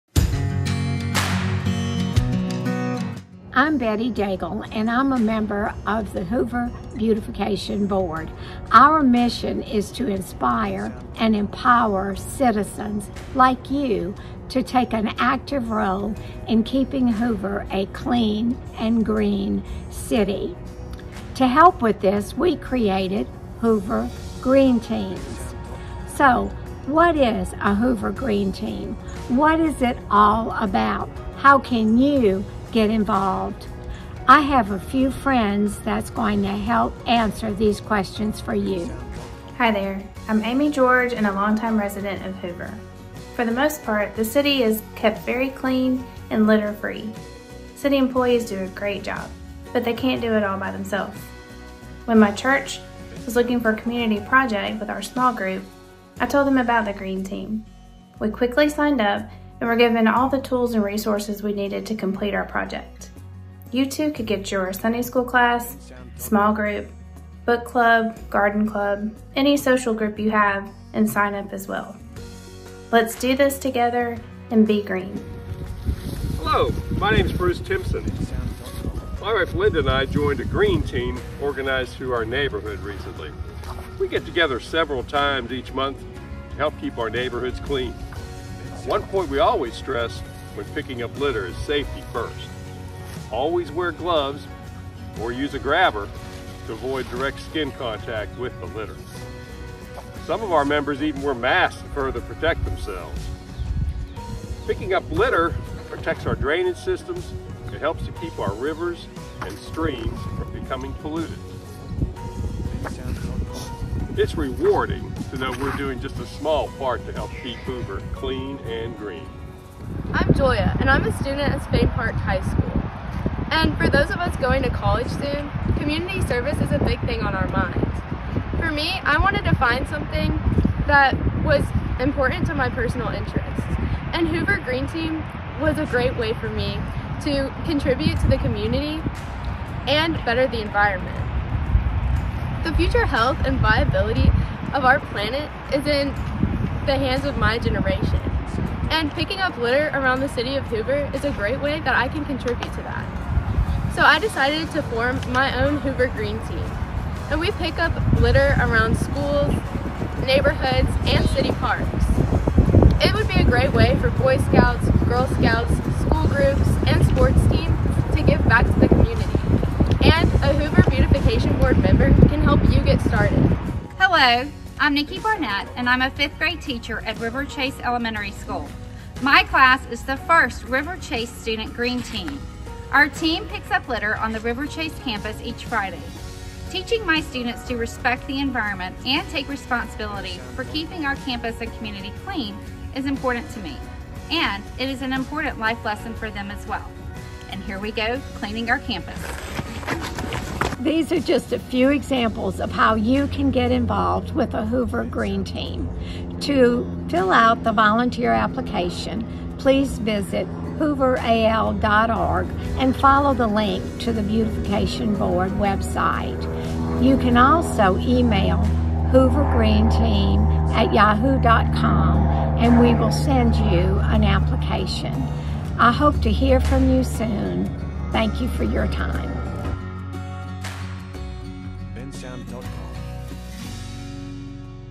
Hoover Green Team Video
In an effort to keep the City of Hoover clean and green, the Hoover Beautification Board has established Hoover Green Teams.  The teams are comprised of volunteers who give of their time to help collect litter from around the City.  They'd love for you to start or be a part of a Green Team.  So how do you get involved.  The video explains it all!